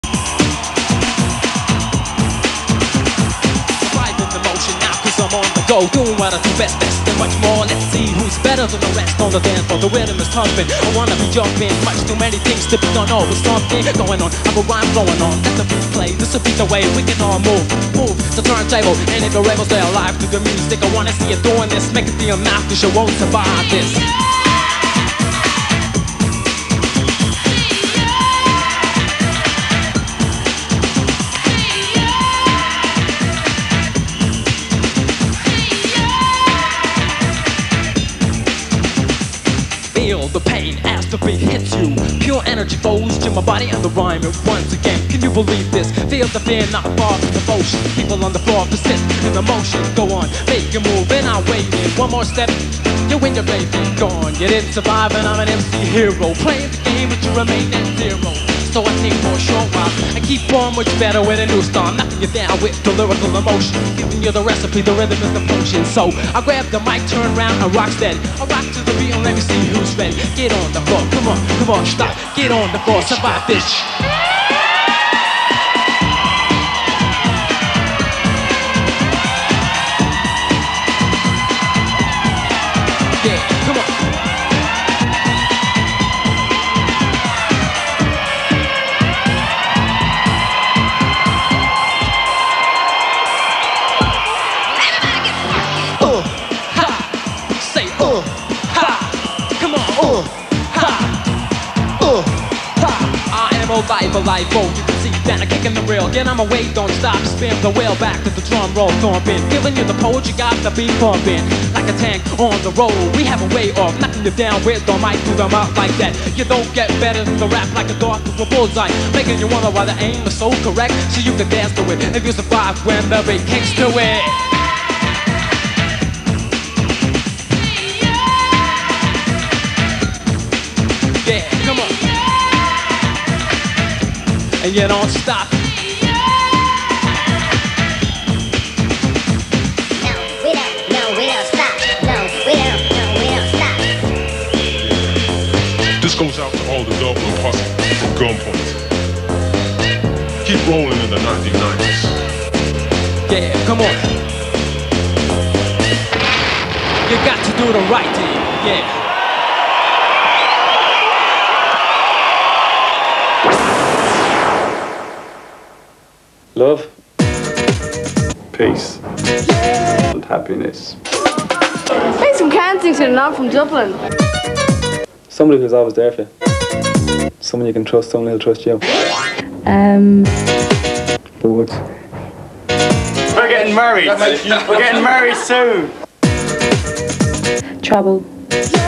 GUN POINT (live) @ fun city 1991
irish rap outfit gunpoint live at funcity, the point depot, dublin. 1991